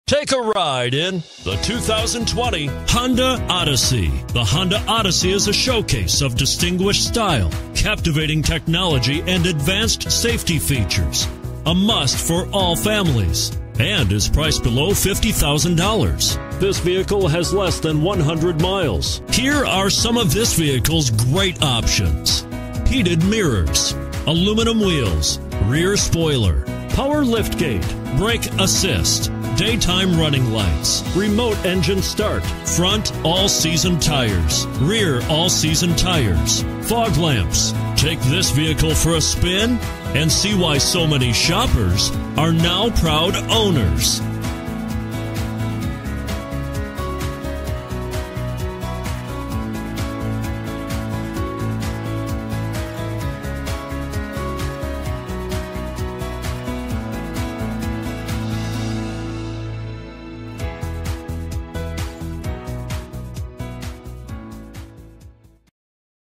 2020 Honda Odyssey AH200351
2020 HONDA ODYSSEY Old Bridge, NJ
Stock# AH200351

BACK-UP CAMERA, LEATHER, HEATED FRONT SEATS, NAVIGATION, LANE DEPARTURE WARNING, BLUETOOTH, MOONROOF, REAR PARKING AID, MP3 Player, BLIND SPOT MONITORING, CHILD LOCKS, 3RD ROW SEAT, KEYLESS ENTRY, 28 MPG Highway, SAT RADIO, POWER LIFTGATE, ALLOY WHEELS.